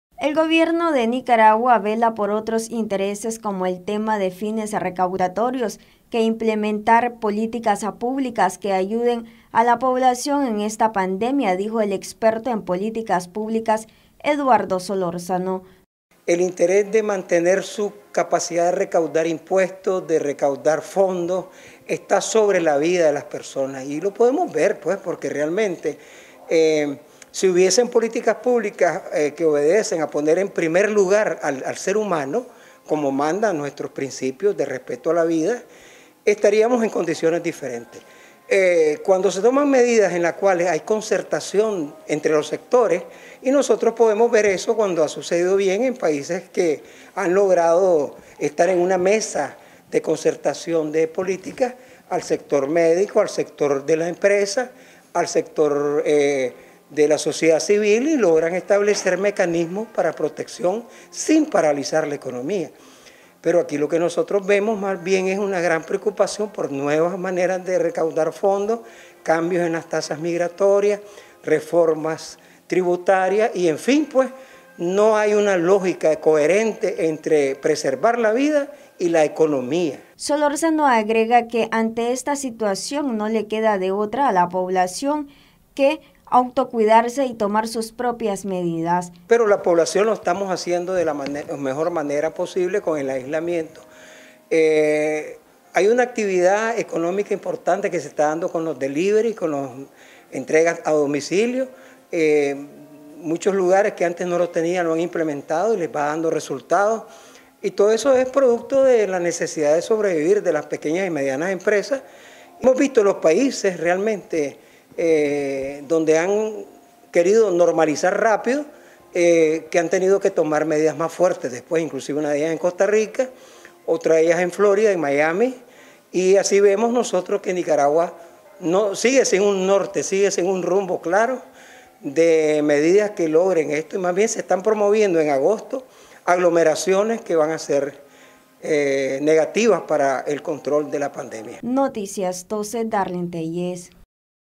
Gobierno tiene falta de creatividad para resolver situación de la pandemia
Experto en políticas públicas afirma que, ante la falta de planes para solventar crisis de pandemia, deja como evidencia que el gobierno vela por otros intereses.